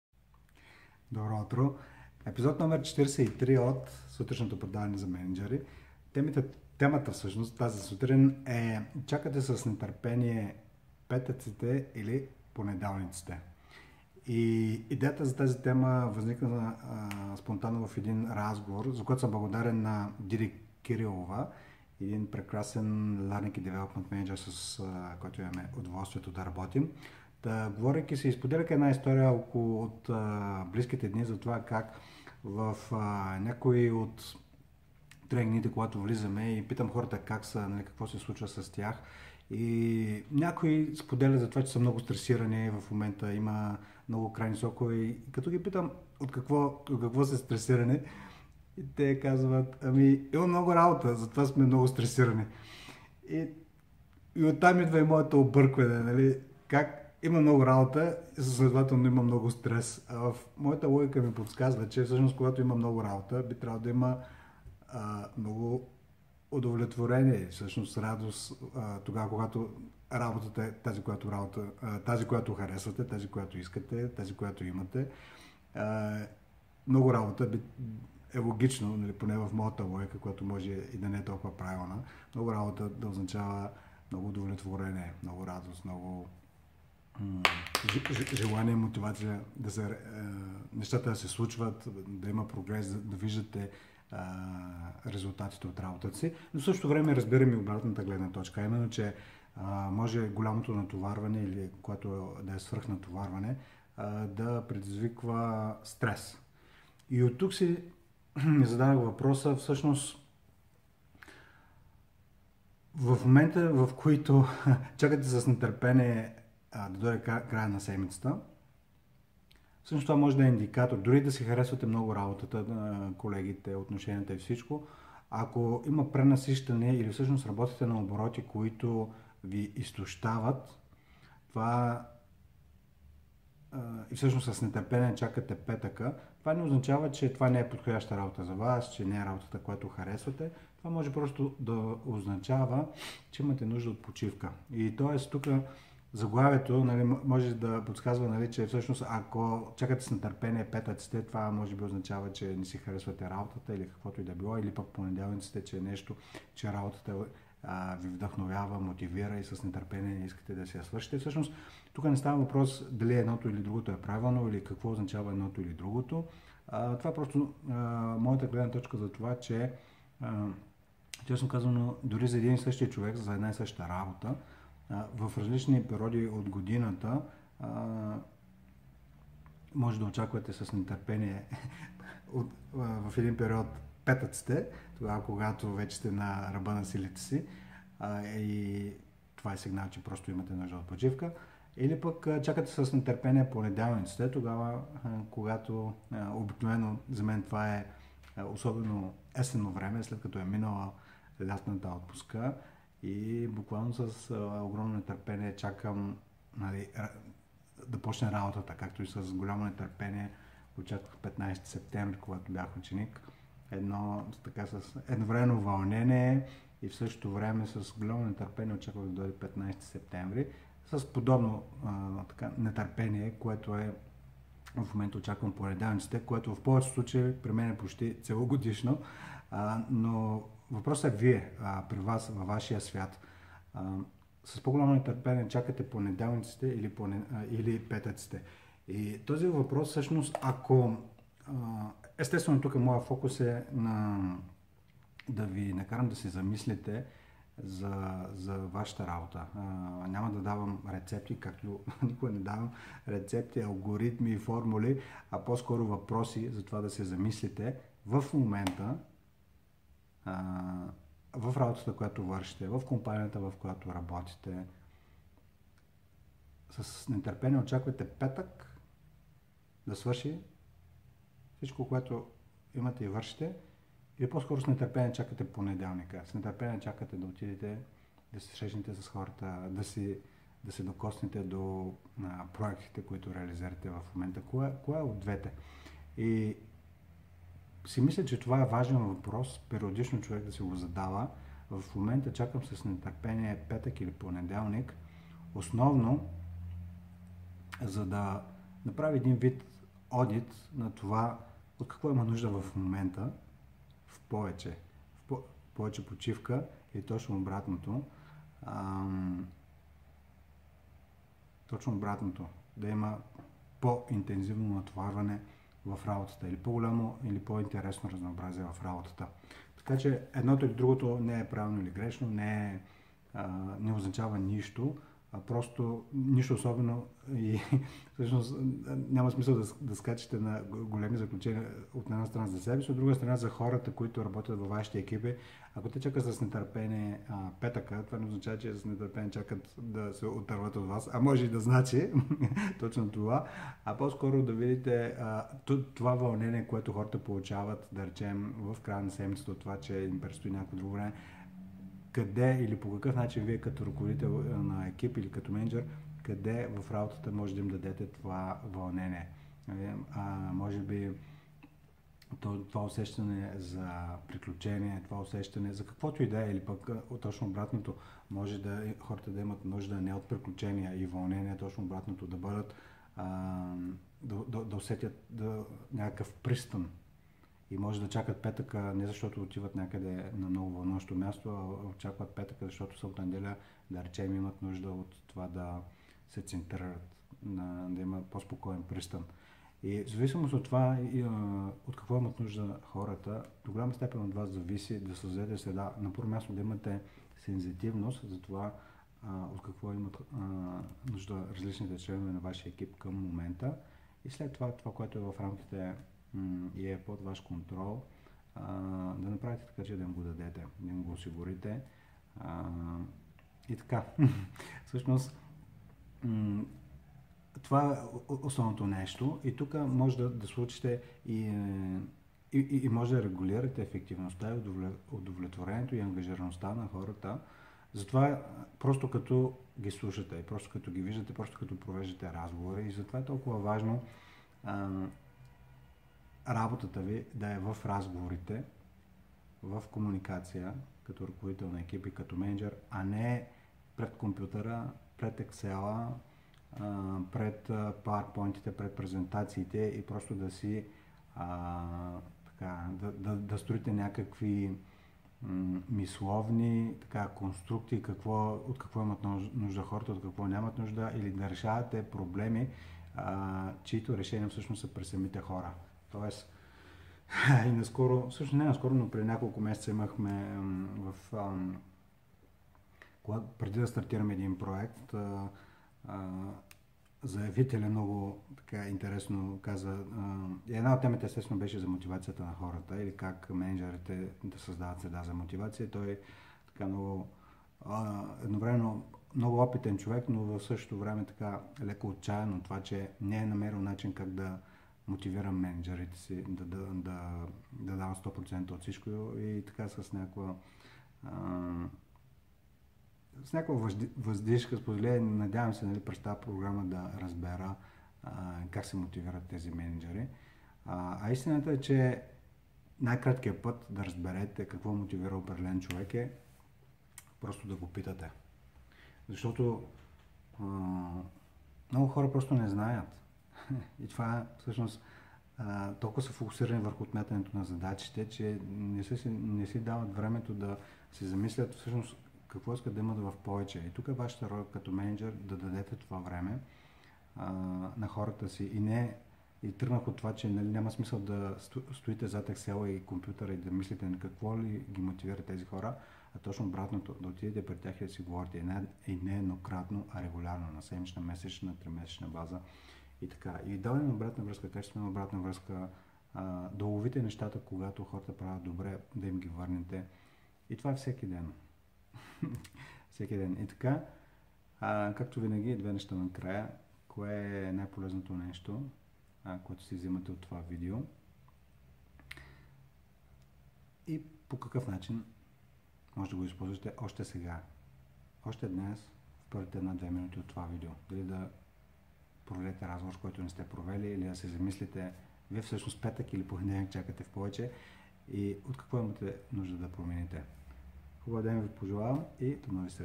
Чакате с нетърпение петък или понеделник? - Епизод 043 на “Сутрешно предаване за мениджъри”
Епизод 043 на “Сутрешно предаване за мениджъри”

Темата: Чакате се нетърпение петък или понеделник?

5-месечната ни програма за развитие на коучинг умения за мениджъри Intunity Coaches стартира на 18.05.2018 в София